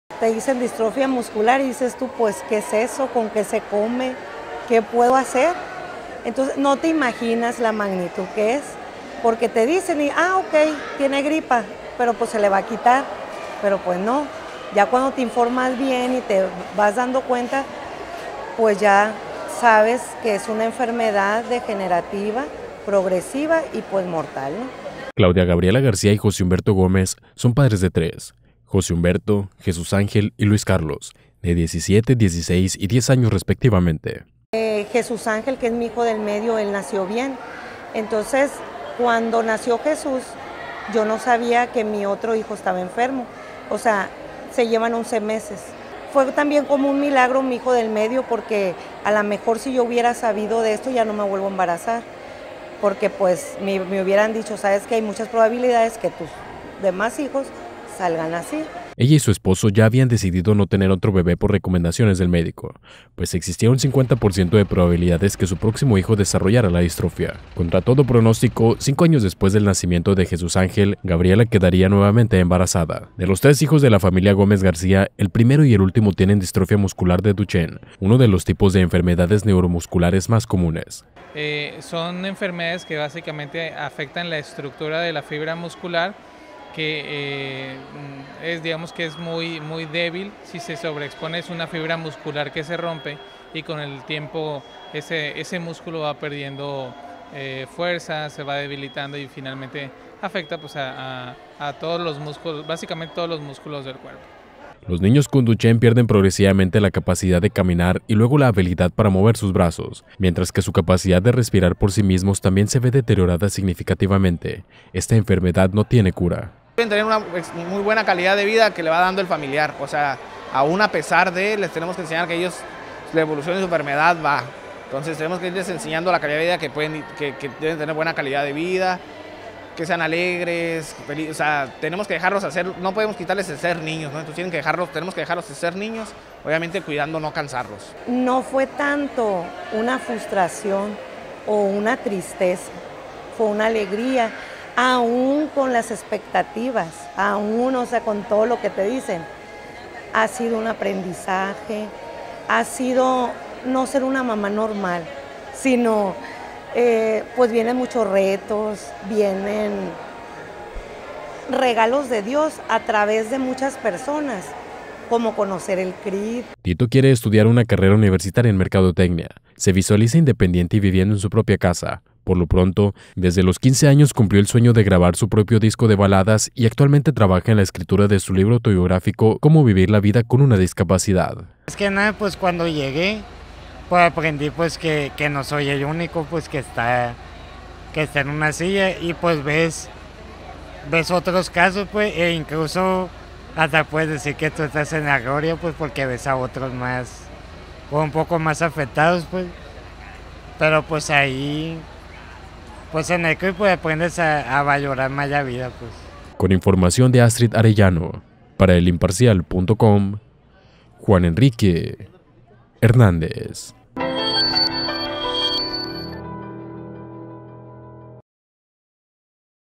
Superan diariamente los retos de su propio cuerpo
“¿Distrofia muscular?”, se preguntó la madre al escuchar al médico, “pues ha de ser como una gripe, ¿no? Se le va a quitar”, se dijo Gabriela mientras observaba a su primer hijo, José Humberto, de apenas dos años, sin imaginar la magnitud del reto al que estaba por enfrentarse.